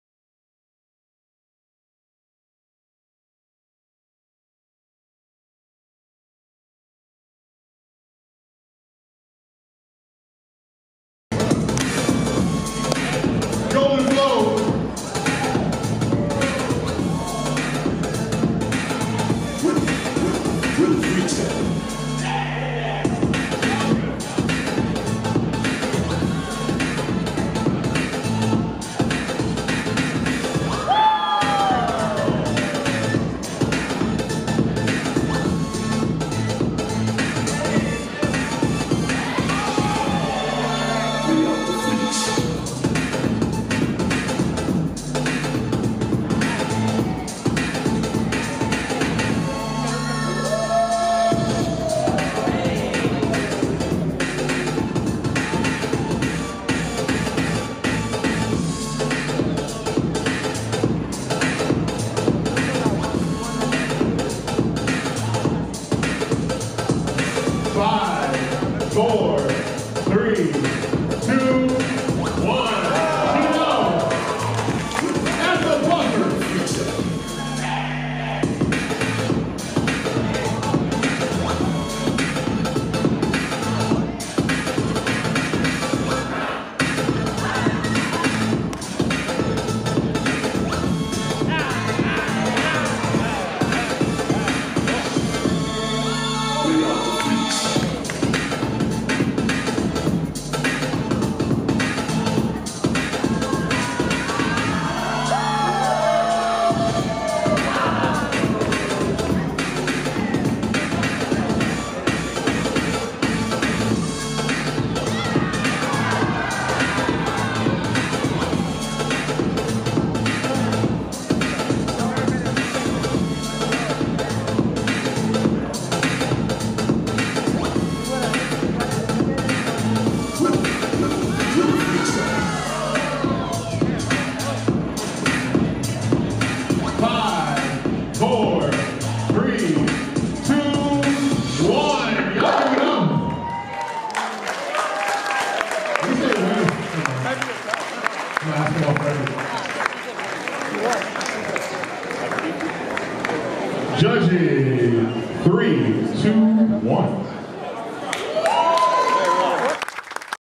Transformations Winter 2023 (Top 8) Goldenflow vs Geno
Location: Westfield Wheaton Mall

Prizes:
1st: $500
2nd: $100 & UADA Semester Class Pass

Partner: @westfieldwheaton
Organizers: @urbanartistrydc